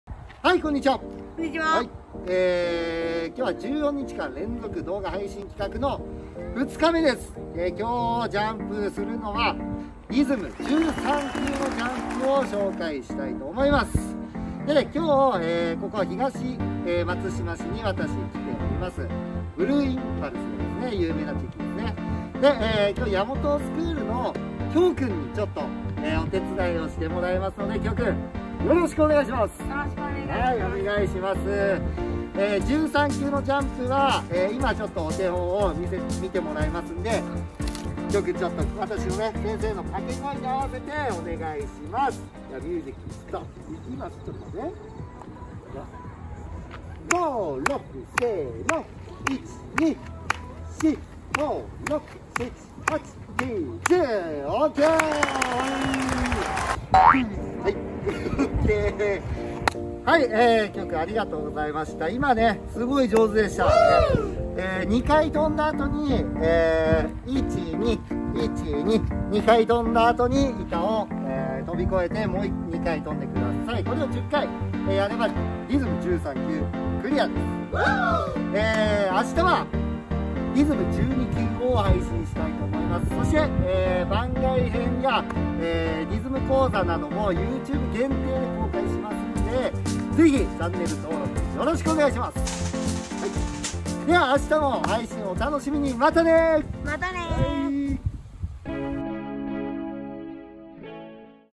【13級ジャンプ】14日間連続リズムジャンプトレーニング動画配信企画！2日目
リズムに合わせて楽しくジャンプしよう！
リズム感を向上して運動神経を鍛えましょう！

徐々に難しくなってきますよ〜！